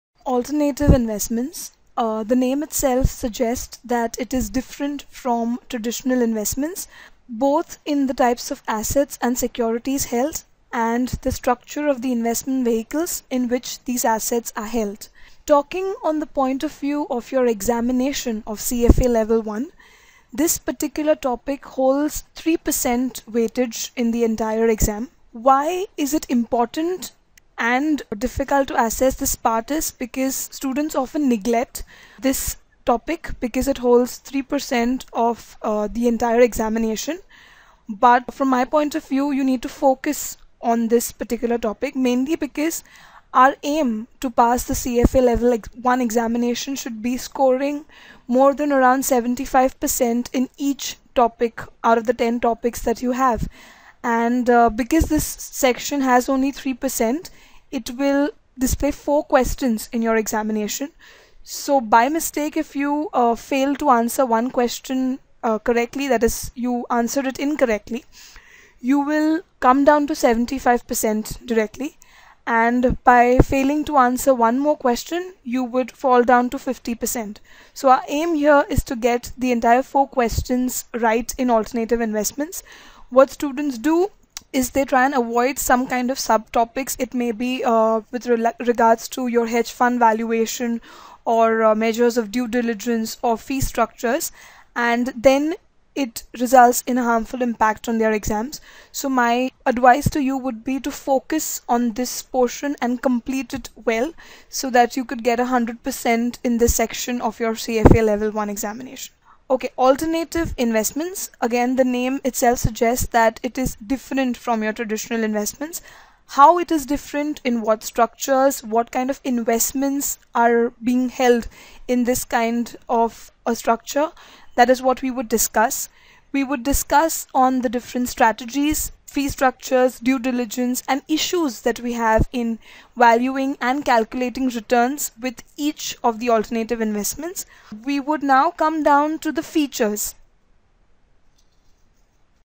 CFA Level I - Alternative Investment
This module of CFA Level I Preparatory guide covers one of the most important part of CFA Level I Exam "Alternative Investments". This module has a weight-age of 3% in the Level I Exam and 5-15% in Level II and Level III exams of CFA. Some participants find it very easy and some of them find it really difficult